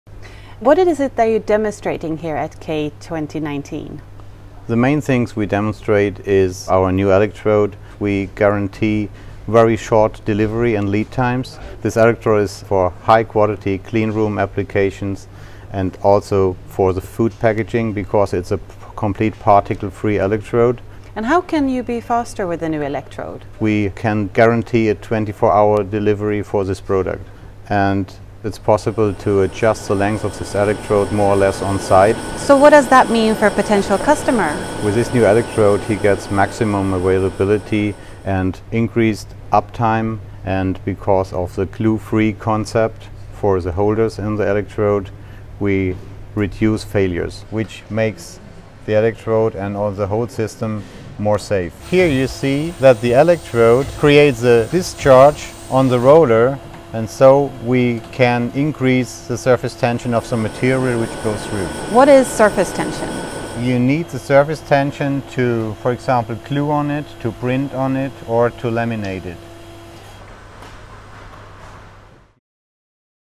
Particle-free Ahlbrandt Corona electrodes
The new particle-free Ahlbrandt Corona electrodes prevent contamination from damaged or broken elements, an innovation that provides an advantage for clean rooms and those converting high quality, food-grade plastic films. Ahlbrandt, a Baldwin Technology company, is well known for its state-of-the-art, high-efficiency Corona systems designed for superior surface treatment. With the new particle-free electrodes, you will not only get a safe and clean manufacturing process, but smart electrode parts that are easily replaced into existing Corona systems.